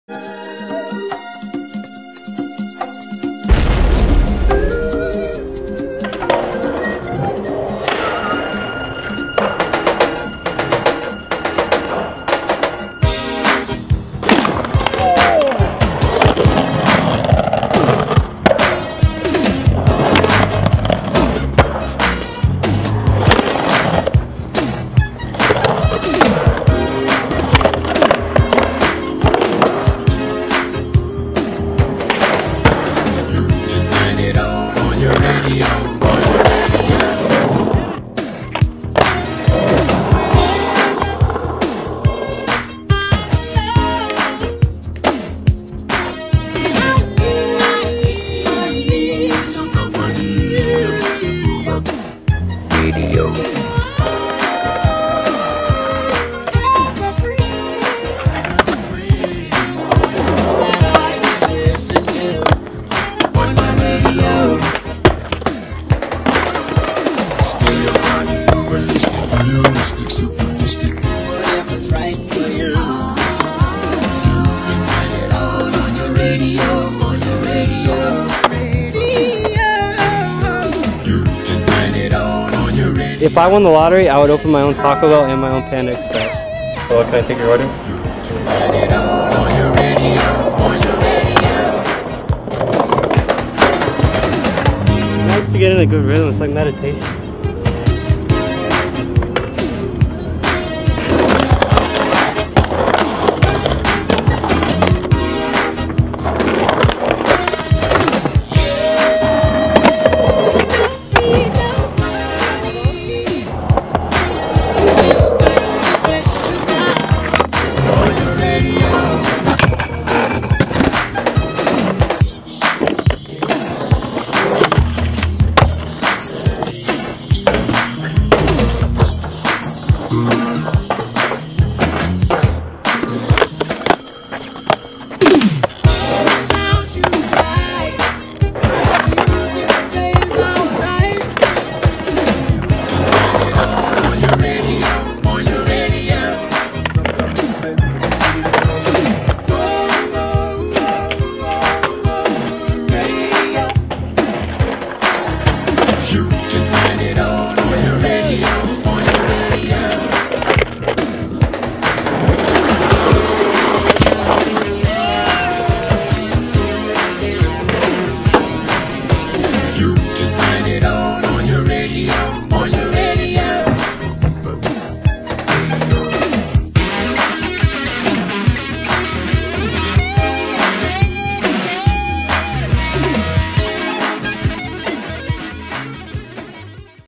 John Benton - SEA to LAX
When I approached John Benton to do a little part to promote some stupid board we just made, I had no idea it would turn into 3 minutes of footage. But when you're just out skating, relaxing and having fun, you tend to end up getting a ton of clips. Which is totally what happened in this case. John flew from Seattle to LA for one day on 3 separate occasions to film this thing, and I couldn't be happier with what we made. Thank you for being you, John. It's always a pleasure pointing the camera in your direction. —Tim Olson